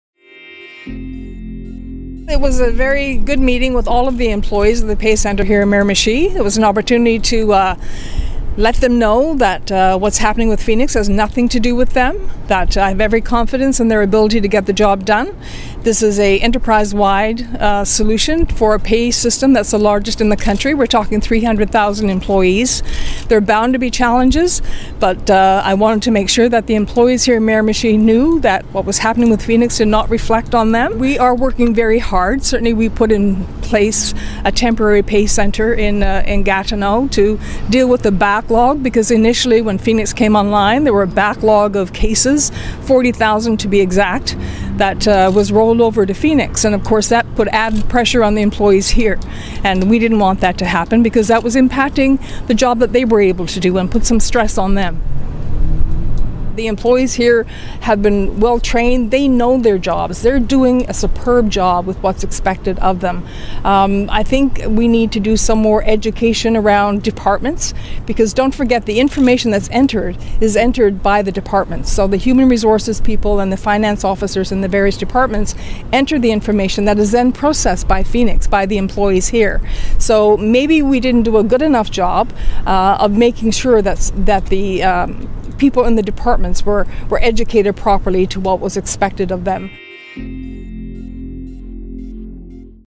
Federal departments share blame for payroll woes: Minister
Public Services Minister Judy Foote says training issues in federal departments may be partly to blame for problems with the government's new Phoenix payroll system. Foote toured the centralized New Brunswick pay centre on Wednesday. (July 27, 2016)